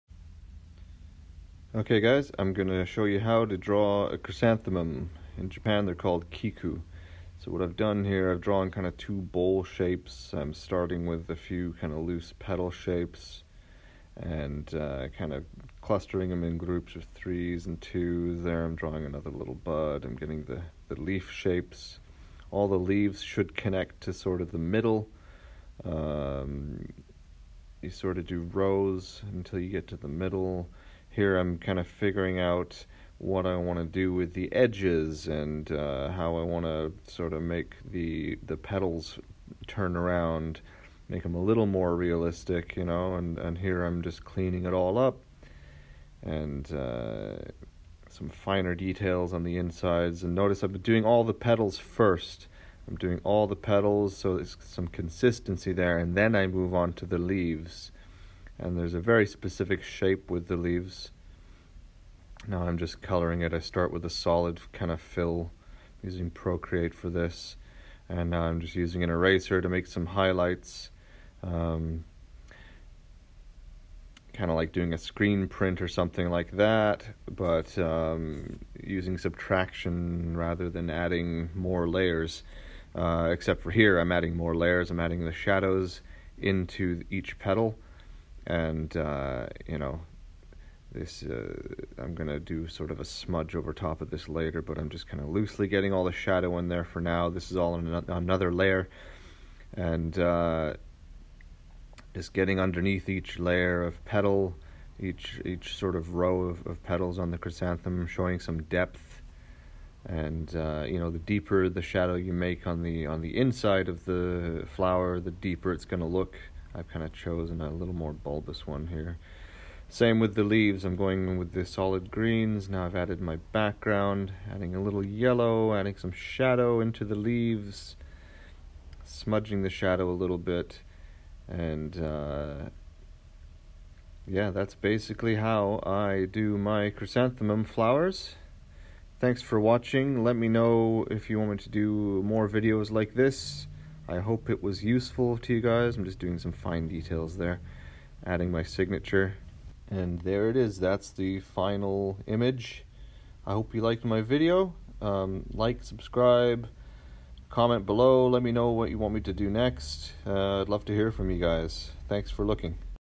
How to draw chrysanthemum or Kiku Japanese tattoo design
Ninjamie shows you how to draw chrysanthemum flowers.  Most difficult flower to draw. In Japan this flower is called Kiku. Step by step tutorial.